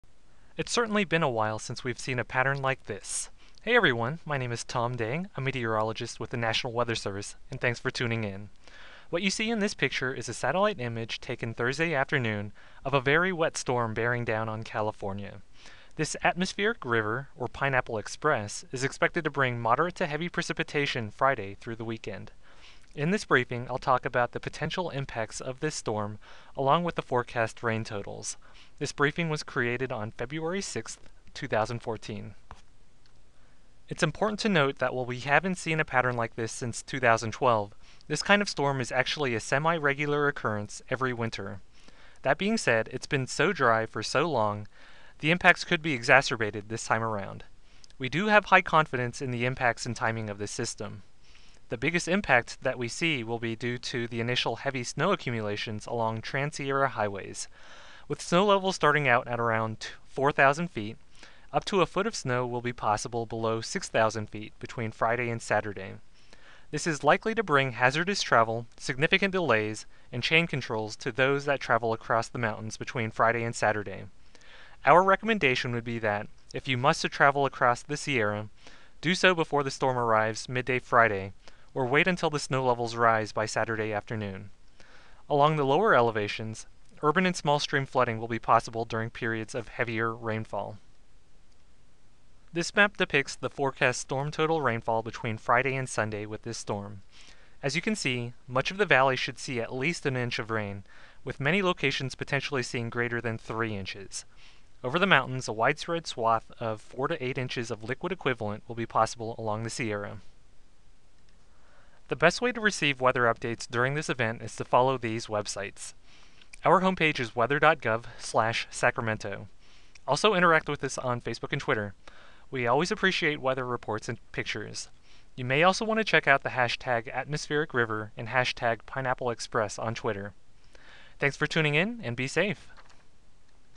An Atmospheric River is Approaching California! Here's What You can Expect
The National Weather Service in Sacramento is providing you the details on impacts and forecast totals with the upcoming Atmospheric River event.